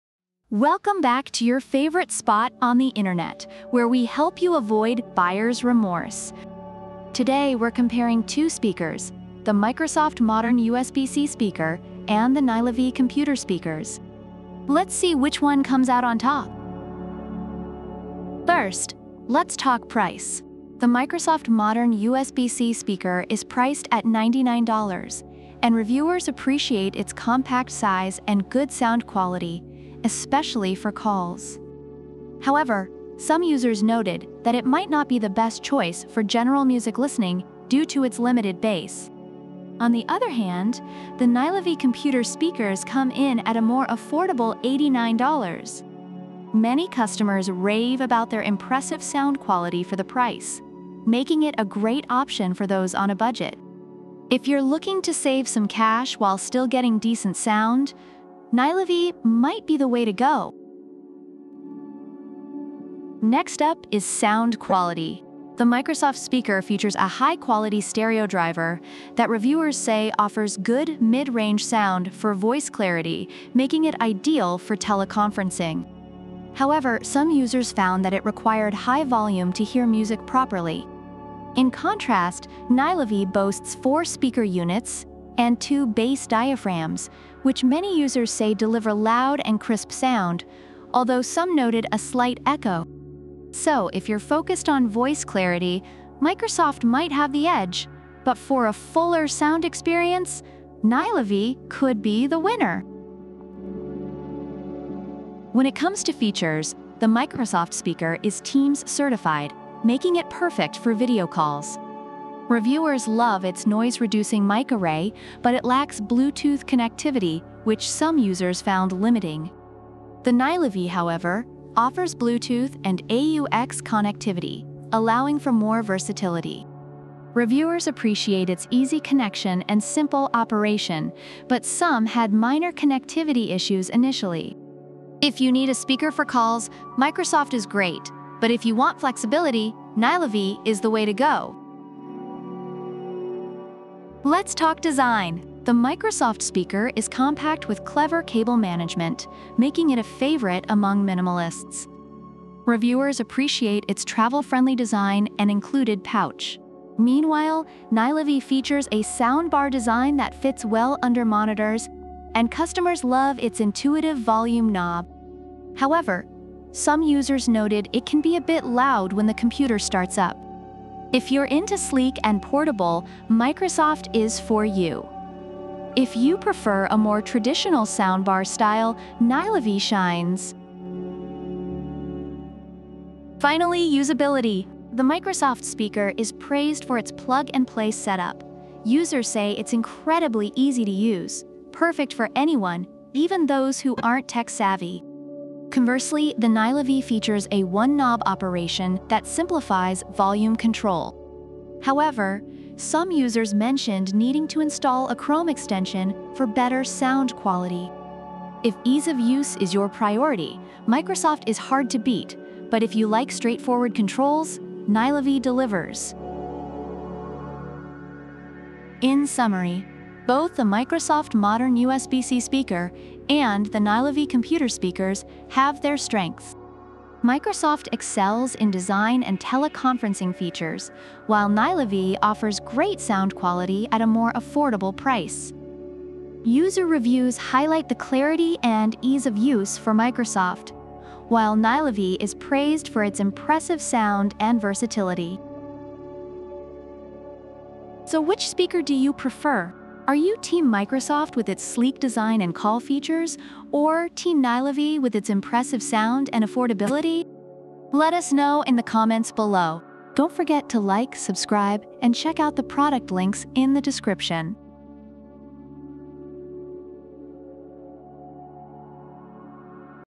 Microsoft Modern USB-C Speaker vs Nylavee Computer Sound Bar Comparison 🎧🔊
In this video, we compare the Microsoft Modern USB-C Speaker and the Nylavee Computer Sound Bar. Check out the Microsoft Modern USB-C Speaker
Discover the features, sound quality, connectivity options, and unique advantages of each product to help you make an informed decision on the ideal computer speaker for your setup and needs. 🎵🔊📱💻🎮